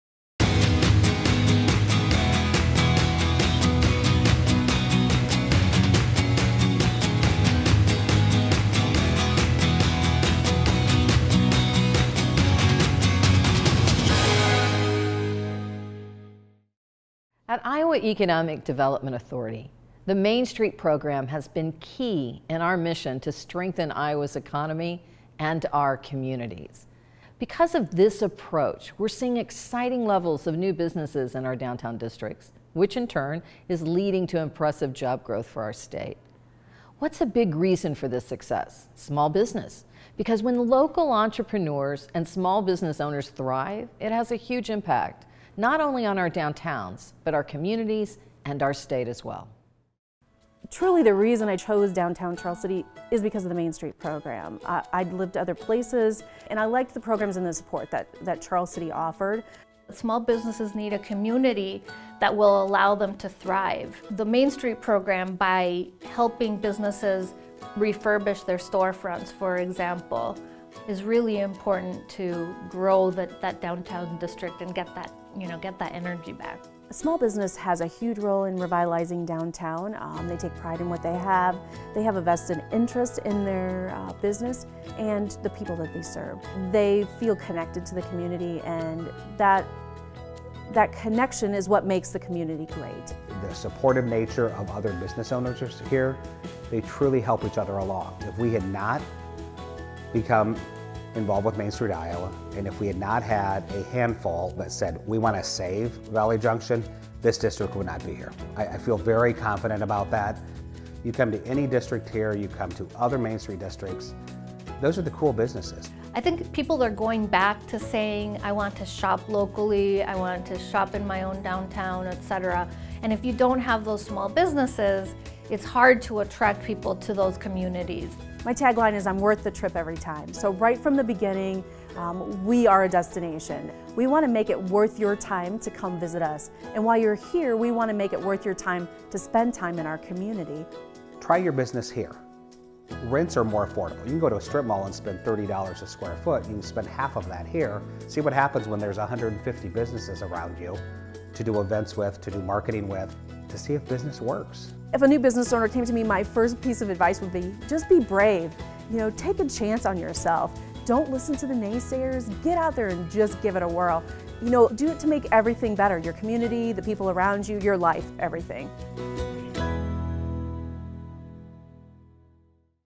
Main Street Iowa: Entrepreneurship
At the Iowa Economic Development Authority, our Main Street program has been key in our mission to strengthen Iowa’s economy and communities.  It’s transforming our downtown districts by supporting small businesses and helping them thrive.  The Main Street Iowa network has created a net gain of over 4,300 new businesses, employing more than 12,800 new downtown employees.  On Main Streets all across Iowa, our approach focuses on creating an environment where small businesses and entrepreneurs prosper. Because their success has a big impact on our downtowns by re-energizing and strengthening our communities.

Since 1986, Main Street Iowa communities have been improving the economic, physical and social values of their historic Main Street districts. Main Street Iowa provides a framework for empowering local leaders to engage with their community in a dialogue to reach consensus for what's needed and what can be done to return historic commercial districts to their rightful place as economic and cultural centers.